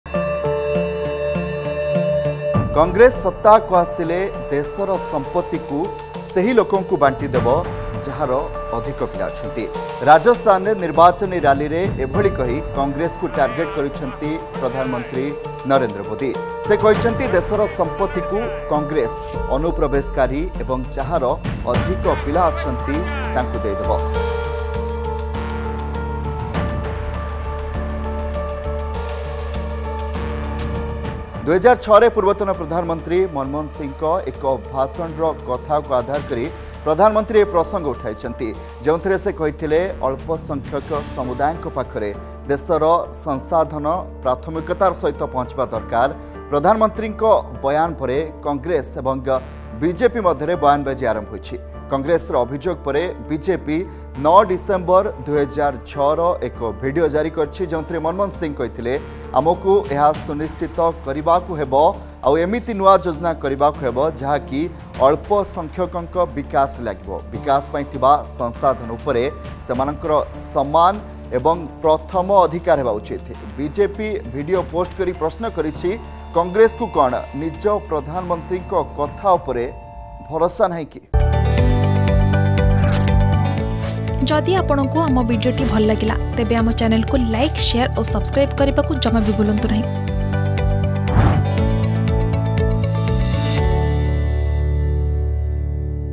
କଂଗ୍ରେସ ଆସିଲେ...ଦେଶର ସଂପତ୍ତି ତାକୁ ଦେବ, ଯାହାର ଅଧିକ ପିଲା ! || Congress || Narendra Modi
କଂଗ୍ରେସ ସତ୍ତାକୁ ଆସିଲେ, ଦେଶର ସଂପତିକୁ ସେହି ଲୋକଙ୍କୁ ବାଣ୍ଟିଦେବ, ଯାହାର ଅଧିକ ପିଲା ଅଛନ୍ତି । ରାଜସ୍ଥାନରେ ନିର୍ବାଚନୀ ରାଲିରେ ଏଭଳି କହି କଂଗ୍ରେସକୁ ଟାର୍ଗେଟ କରିଛନ୍ତି ପ୍ରଧାନମନ୍ତ୍ରୀ ନରେନ୍ଦ୍ର ମୋଦି ।